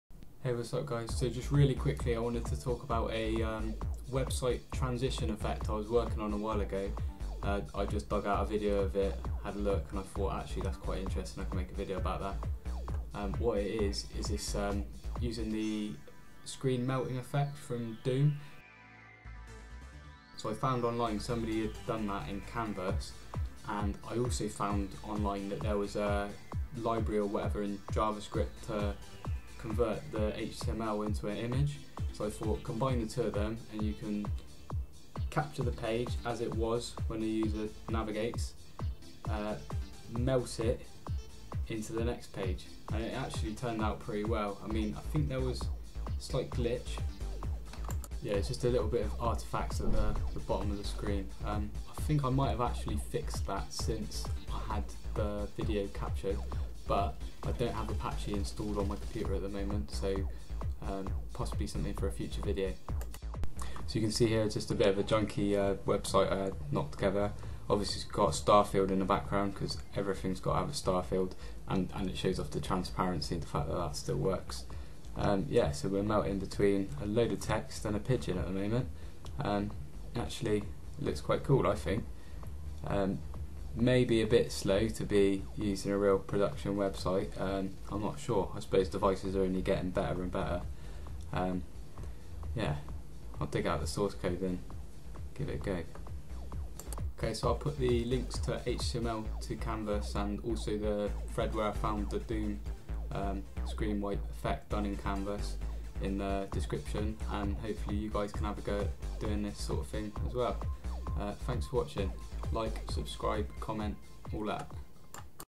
DOOM screen melt effect for website transitions (HTML 5 Canvas)
Just a short video where I talk about an old capture of a website transition effect I found on my old harddrive.

I still have the code, but it had php involved for fetching the next html (not that this is neccessary for this effect), so I cannot run it again without installing apache.

Hopefully this is interesting to someone!

Here is the blog post where i found the canvas implementation of the Doom screen melt